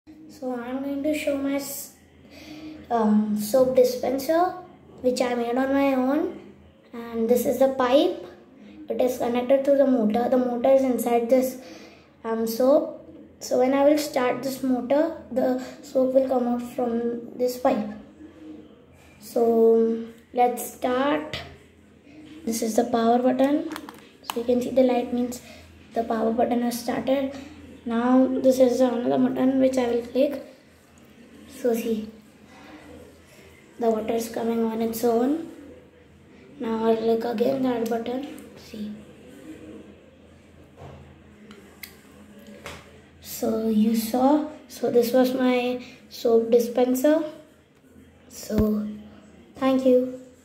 Soap Dispenser
Name: Soap Dispenser

Components: Water Pump, 9V Battery, Switch and Pipe